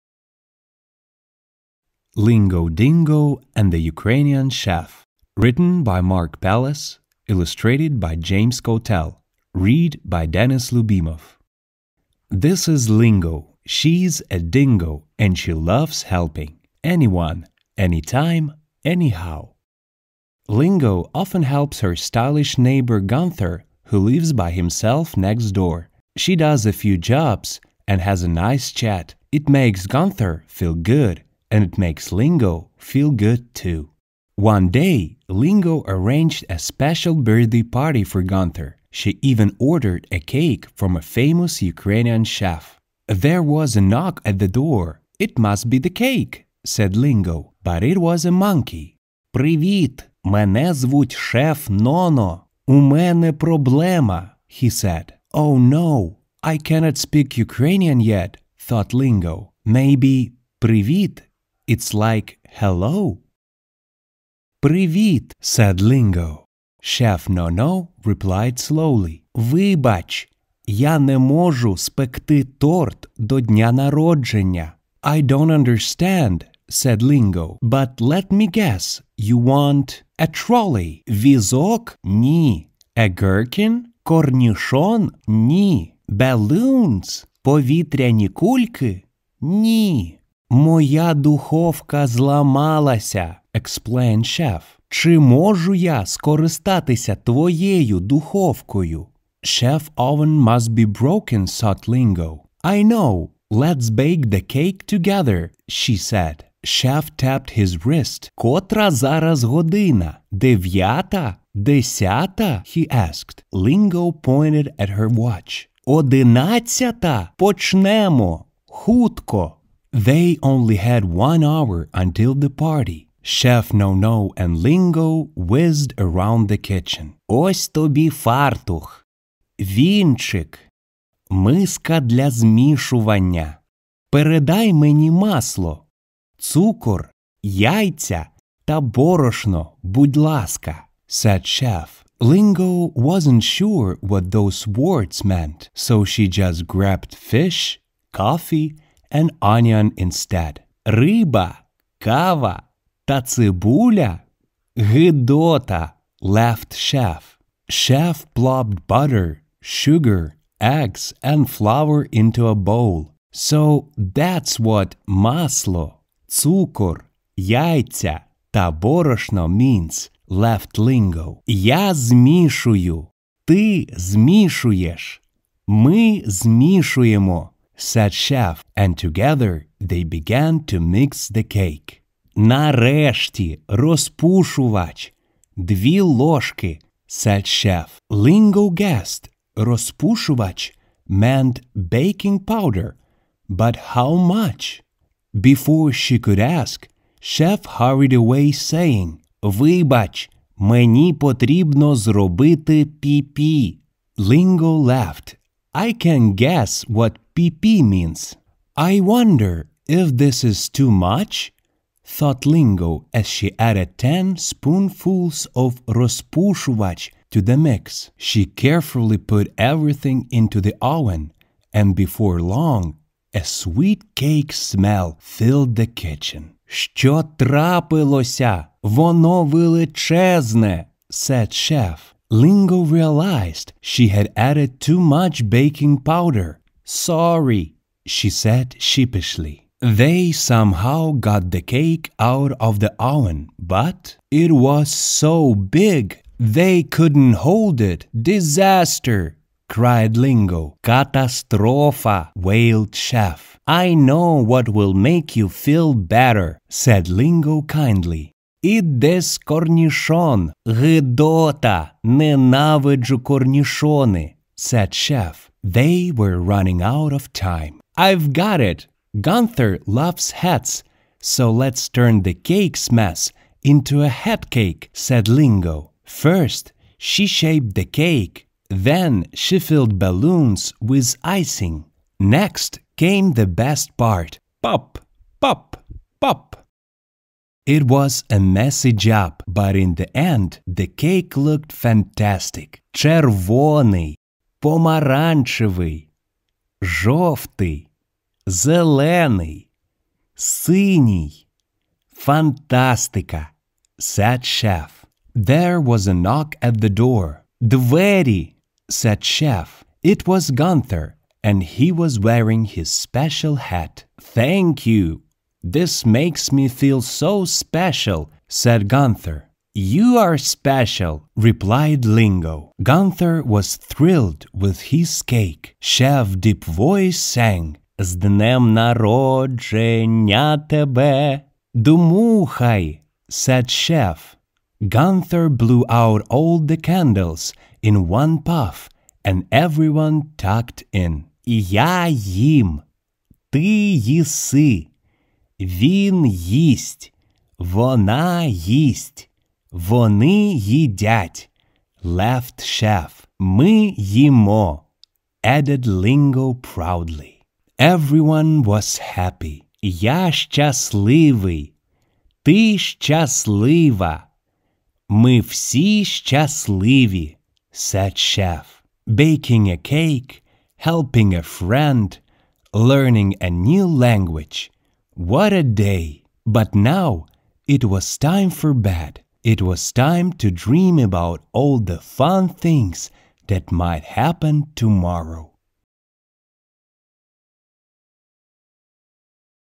Lingo Dingo and the Ukrainian Chef, by Mark Pallis. Learn Ukrainian for kids
For those people who want to learn Ukrainian, especially Ukrainian for kids. Read aloud by Denis Lyubimov, a native Ukrainian speaker, to help teach perfect pronunciation. For more books to help you learn Ukrainian and other languages, Written by Mark Pallis, illustrated by James Cottell